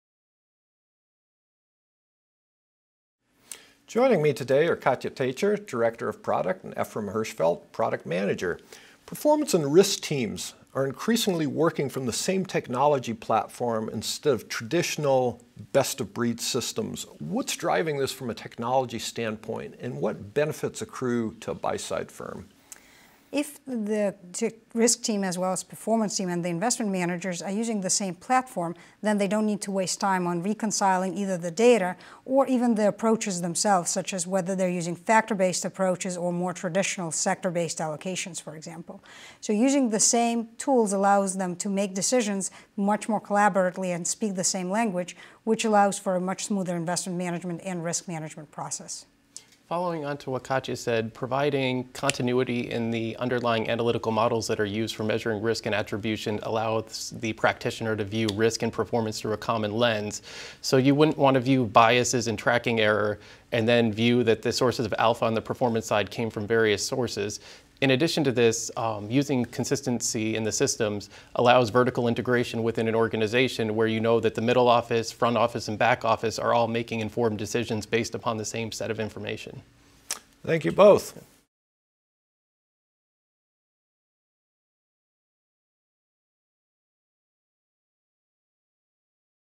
Evaluating Portfolio Risk & Performance on a Single Technology Platform - 2 Minute Take
Working from a single risk and performance platform helps improve collaboration across the front & middle office, and reduces time spent reconciling information from disparate systems. The growing popularity of multi-asset strategies also require performance attribution capabilities across asset classes, enabling investment managers to retire asset class-specific point solutions.